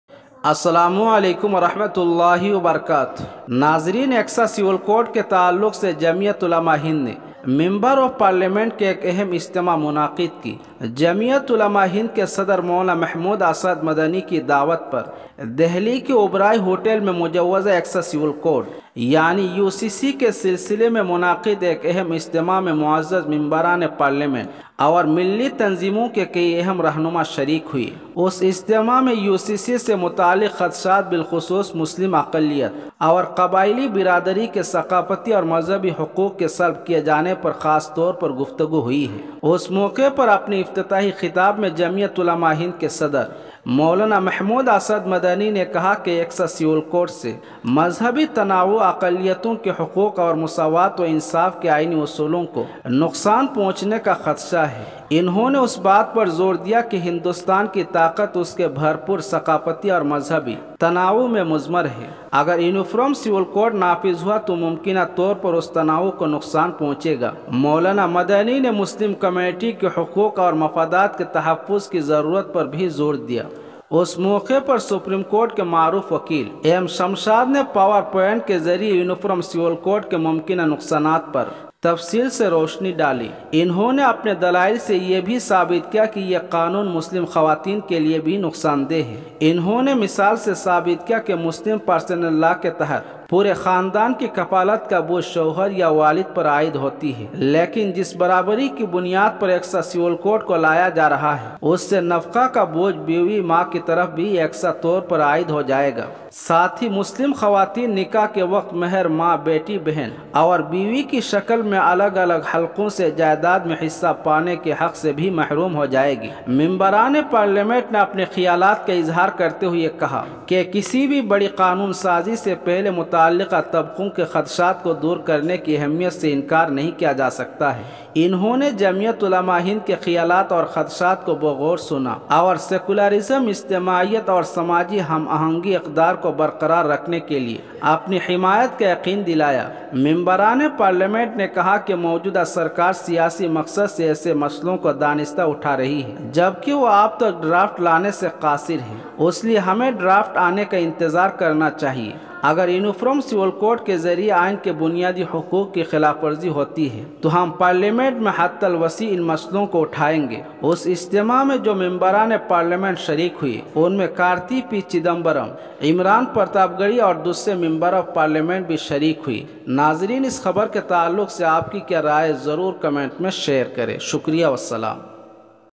July 26, 2023 یونیفارم سول کوٹ کے خلاف جمعیت کا ممبر اف پارلیمنٹ اور رہنماؤں کا اہم اجتماع
یونیفارم سول کوٹ کے خلاف جمعیت کا ممبر اف پارلیمنٹ اور رہنماؤں کا اہم اجتماع
uniform civil code